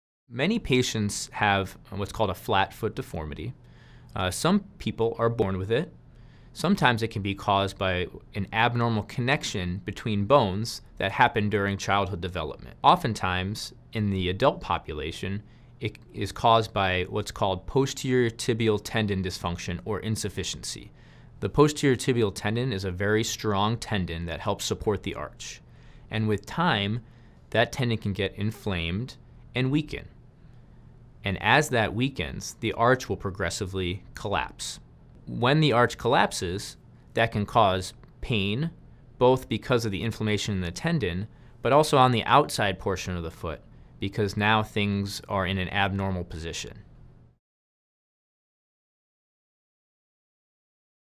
What are flat feet and what causes flat feet? - Reston Hospital Center
Paul Switaj, MD from Reston Hospital Center explains what flat feet are and some of the underlying causes in this video.